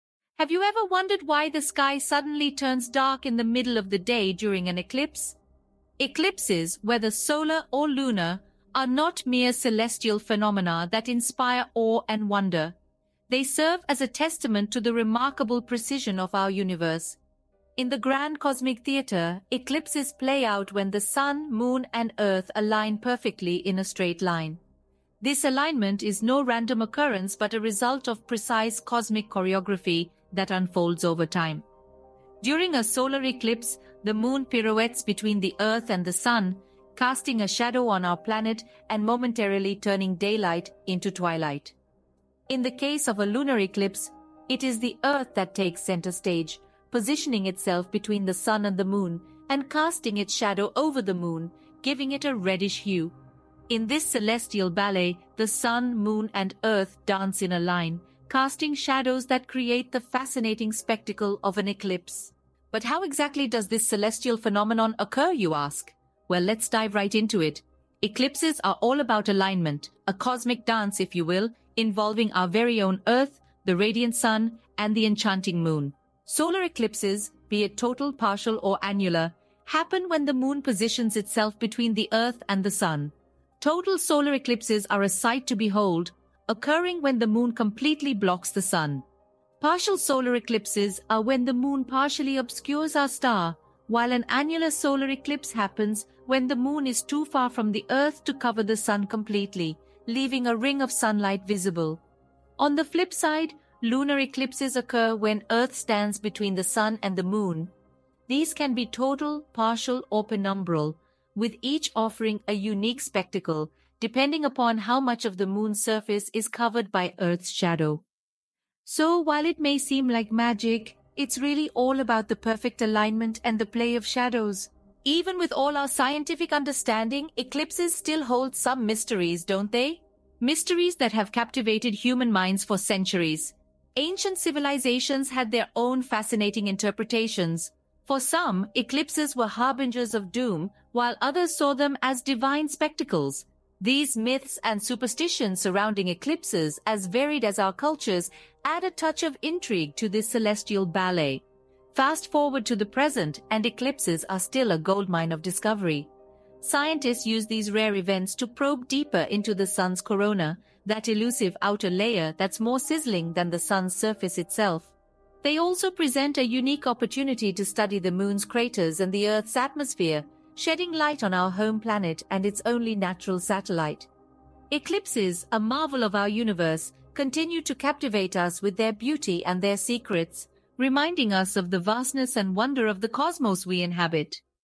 Mysteries of Eclipses | Top 3 Mysteries Unveiled
In this captivating video, join us on a mesmerizing journey through the enchanting world of eclipses. Explore the celestial ballet of these natural wonders as we unravel the mysteries behind their beauty and significance.

Immerse yourself in the wonder of the cosmos with stunning visuals accompanied by a gentle and mysterious soundtrack that will transport you to another realm.

Don't miss out on this unique opportunity to witness the magic of eclipses like never before. Let's embark on this celestial adventure together! ✨🌓🌌

OUTLINE:
00:00:00 The Enigma of Eclipses
00:01:02 The Science Behind Eclipses
00:02:04 The Mysteries of Eclipses